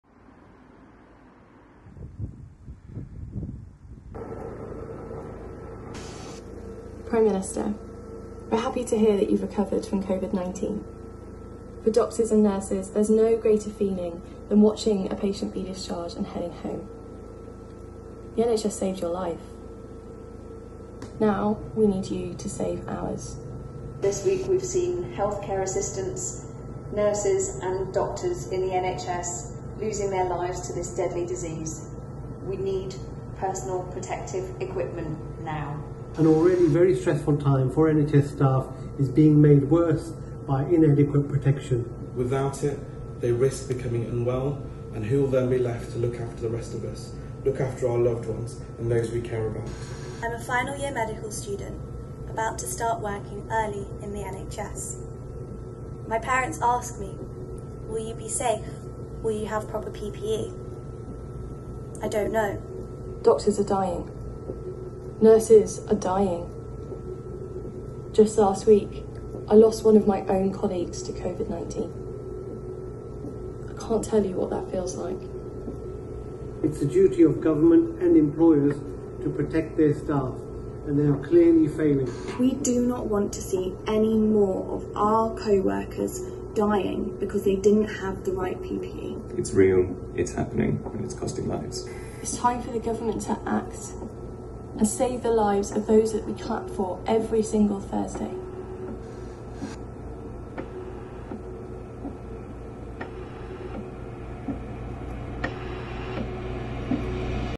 Doctors in powerful message to Boris Johnson on PPE with Led by Donkeys
The Doctors’ Association UK were proud to be asked to front this video from the incredible team at Led by Donkeys. Here our frontline doctors ask Boris Johnson for more PPE in a powerful message to the government “the NHS saved your life...now we need you to save ours”.